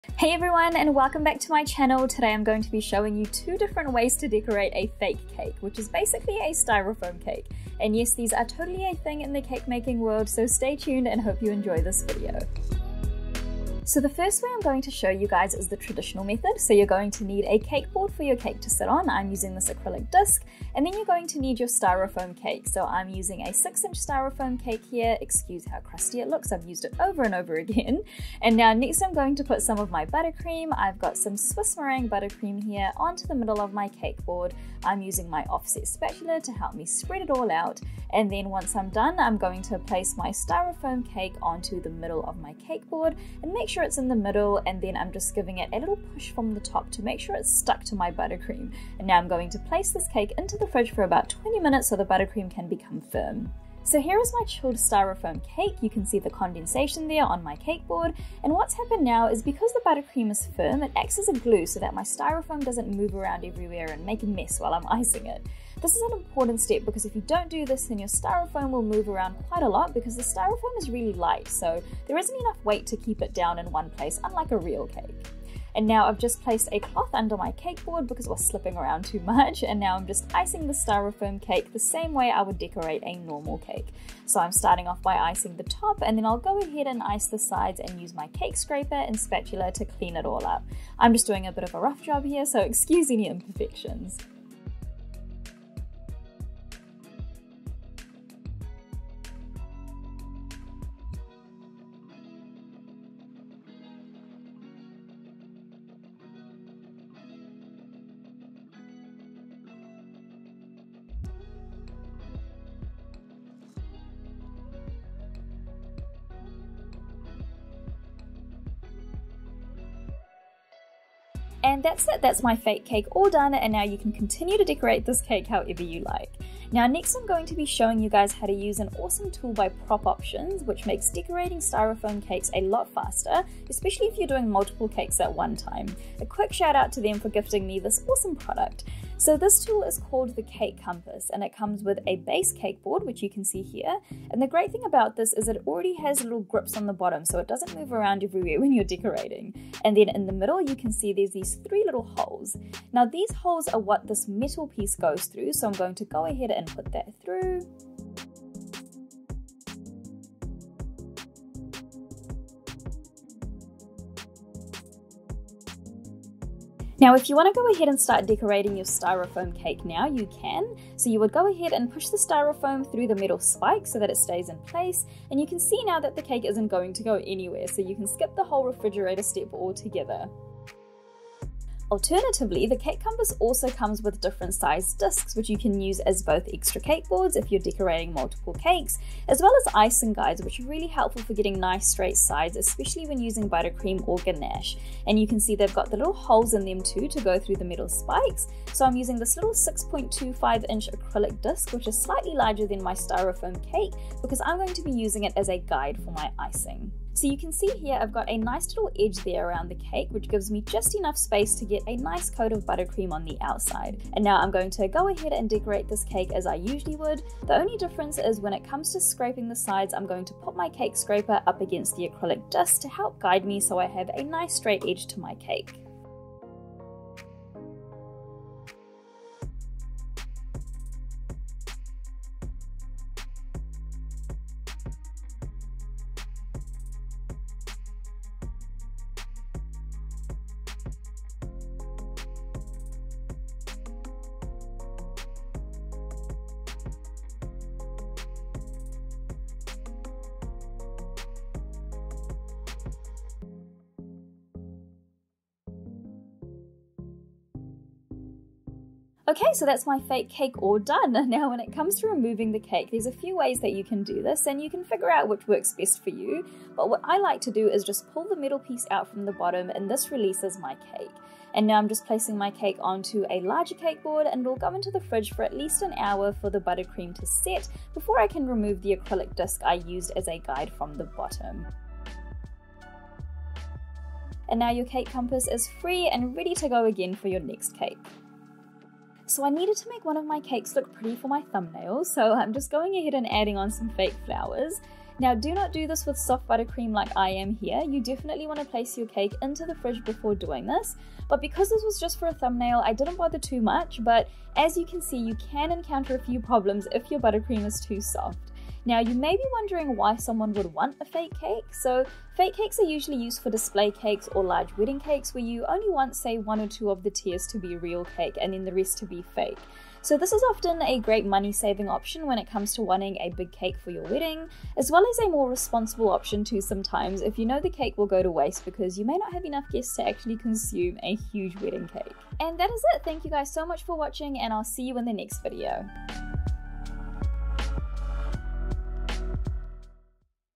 Yes - FAKE CAKES are really a THING! │ Dummy / Polystyrene Cake Tutorial
In today's video I'm going to be showing you all two different ways to decorate a fake cake! Yes - fake cakes are totally a thing, and are great for many uses! These include adding additional tiers to wedding cakes which saves money and reduces cake wastage, making display cakes for shops or trade shows, and for practicing different cake designs on! They're also great for teaching others - I use fake cakes all the time ;)

The second method I demonstrate is using the Cake Compass by Prop Options which was very kindly gifted to me! It's such a great tool and allows you to quickly and efficiently decorate multiple fake cakes at a time. Here's the link if you want to check it out!

OR